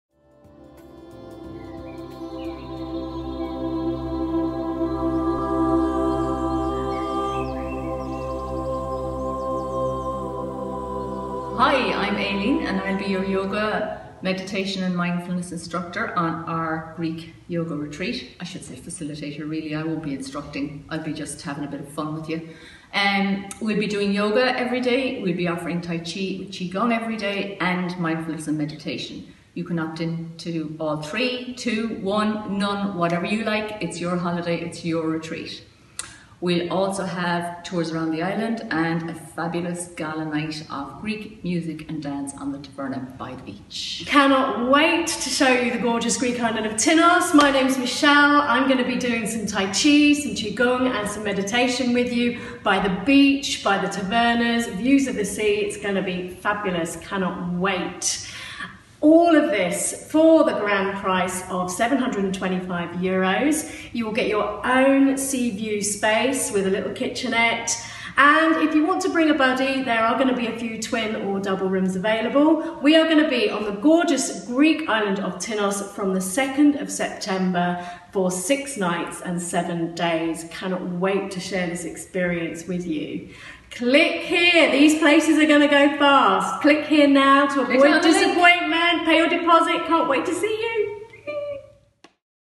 DETAILS!
The details. Yoga & Tai Chi, Sun Sand & Sea, 7-Day Greek Island Retreat 2nd Sept 2018 with Michelle Gupta & Aileen Warbrook! CLICK LINK IN THE TITLE (or below) for ALL DETAILS (Cost and what's included, travel and location, accommodation etc) and to BOOK YOUR PLACE & PAY YOUR DEPOSIT! Cost €725pp based on single occupancy (€650 sharing a twin or double). Places are limited so pay your deposit only €225 now to avoid disappointment